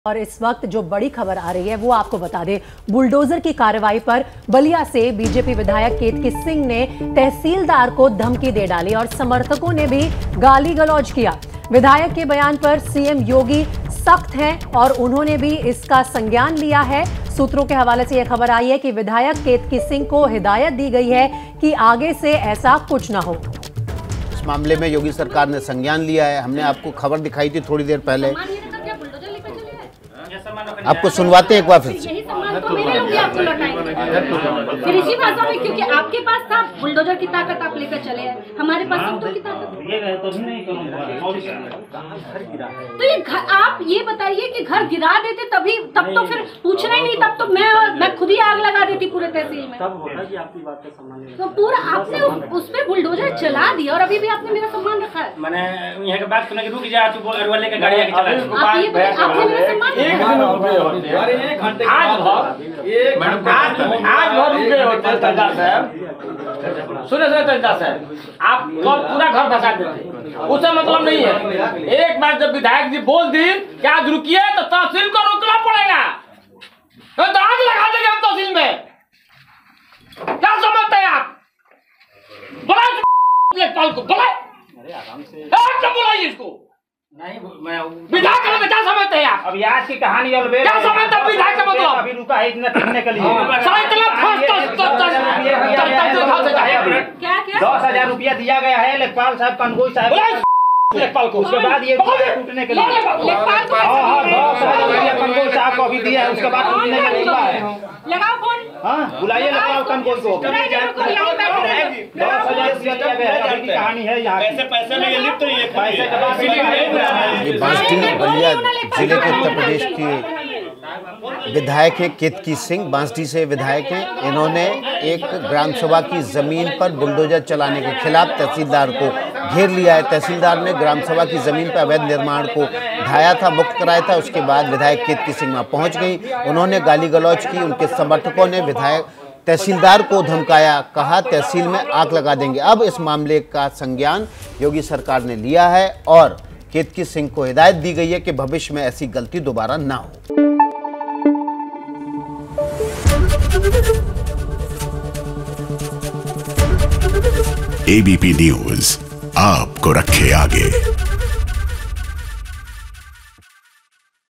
UP News: MLA Ketki Singh has been warned by govt after her controversial remark
HindiNews ABPNews LatestNews UP News: MLA Ketki Singh has been warned by govt after her controversial remark